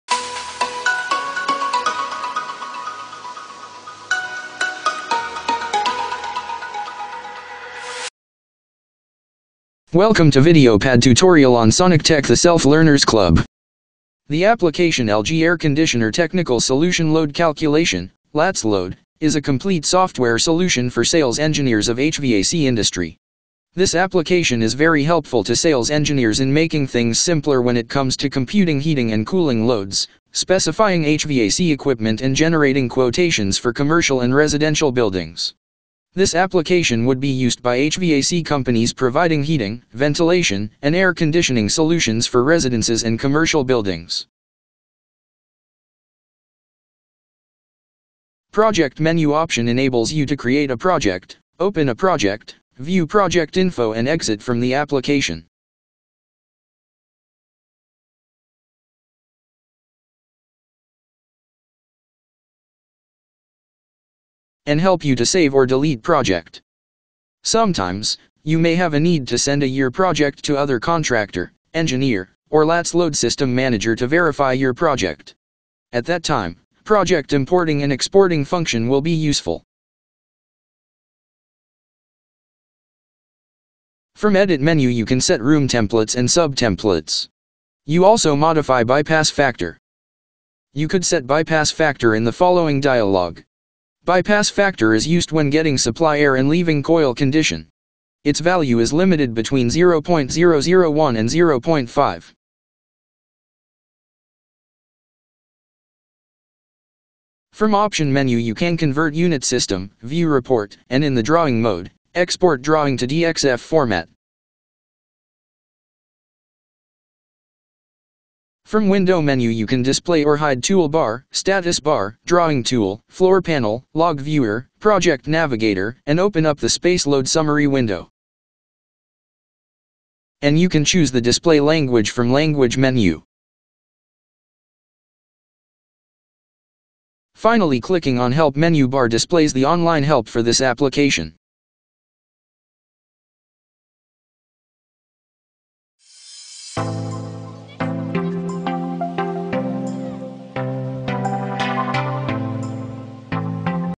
Introduce LATS Load Menu bar - LG LATS Load Tutorial
This video will introduce you to the LATS Load Menu bar.
The application LG Air-conditioned Technical Solution-Load Calculation (LATS-Load) is a complete software solution for Sales Engineers in the HVAC industry.

This application is very helpful to Sales Engineers in making things simpler when it comes to computing heating and cooling loads, specifying HVAC equipment, and generating quotations for commercial and residential buildings.

This application would be used by HVAC companies providing Heating, Ventilation, and Air Conditioning solutions for residences and commercial buildings.

00:00 Intro
00:12 Project Menu
01:23 Edit Menu
01:47 Option Menu
01:58 Window Menu
02:10 Language Menu
02:17 Help Menu
02:26 Outro